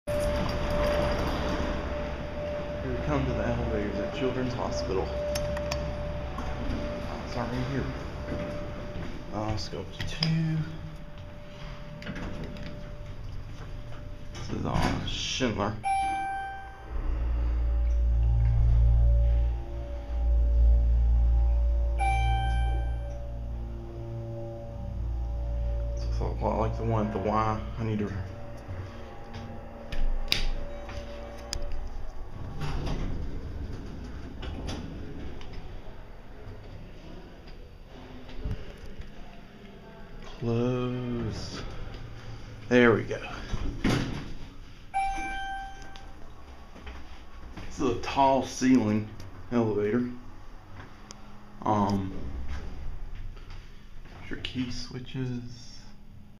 Schindler Hydraulic Elevators at Children's Hospital
Riding the slow hydraulic elevators at Children's Hospital